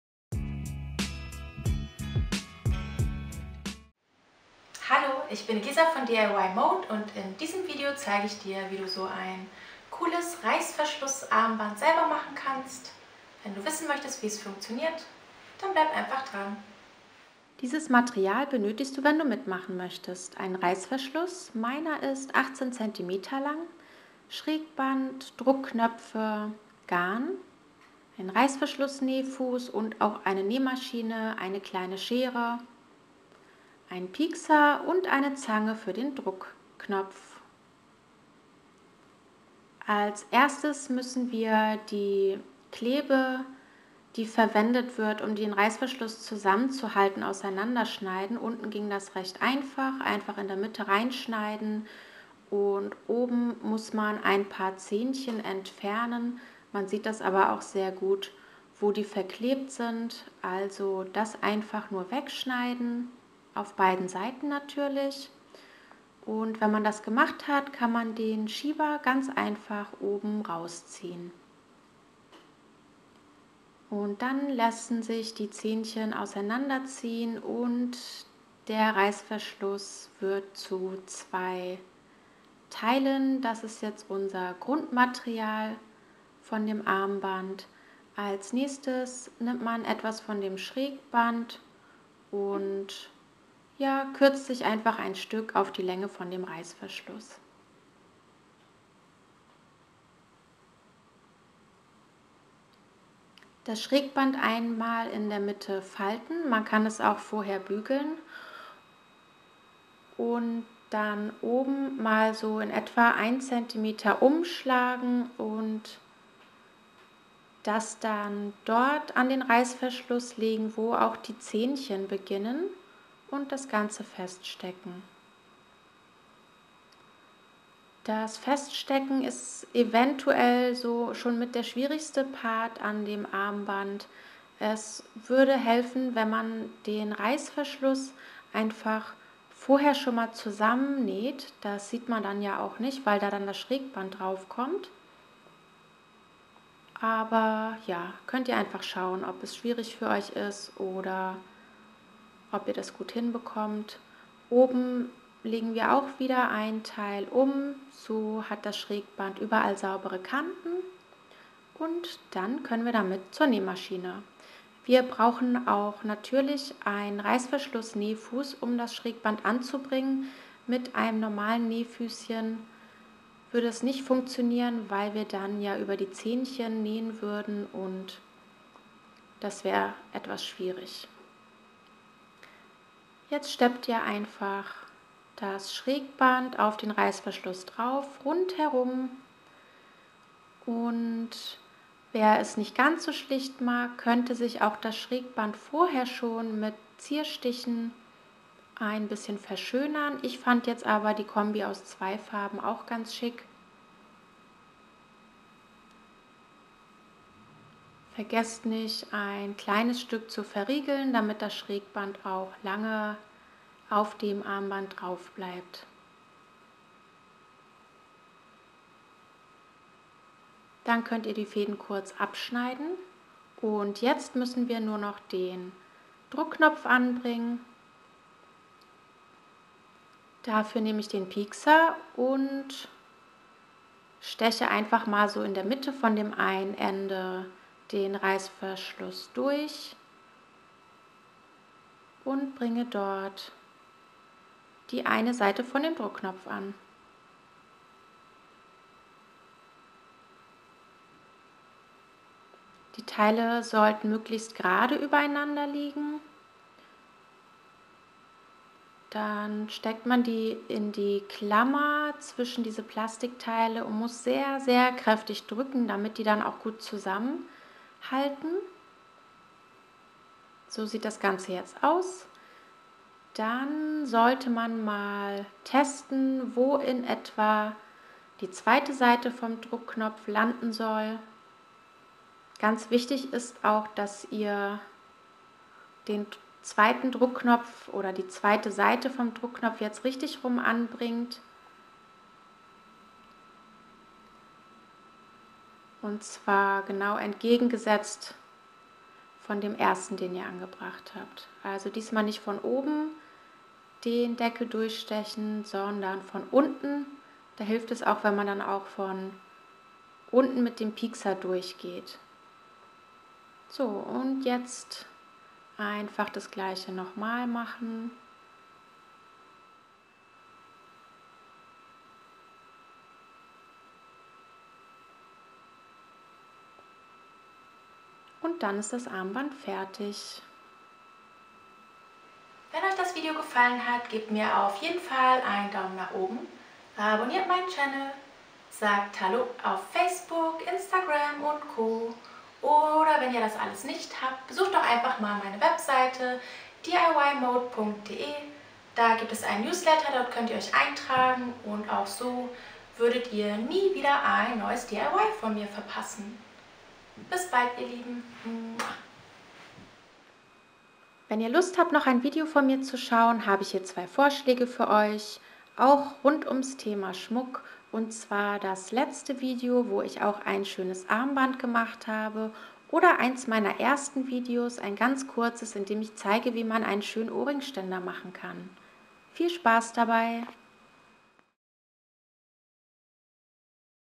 DIY Reißverschluss Armband nähen
In diesem Video zeige ich dir wie du ein cooles Reißverschlussarbmarnd selber machen kannst. Ich wünsche dir viel Spaß beim Anschauen und Nachbasteln.

Material und Werkzeug für dieses DIY:
- Reißverschluss 18-20cm

- Kam Snap Set

- Nähmaschine/Reißverschlussnähfuß

Hier kannst du dich in den Newsletter eintragen wenn du kein DIY mehr von mir verpassen möchtest

Liebe Grüße
Eliza

...und wenn du eine Anleitung von mir umgesetzt hast, schick mir mal ein Foto :-)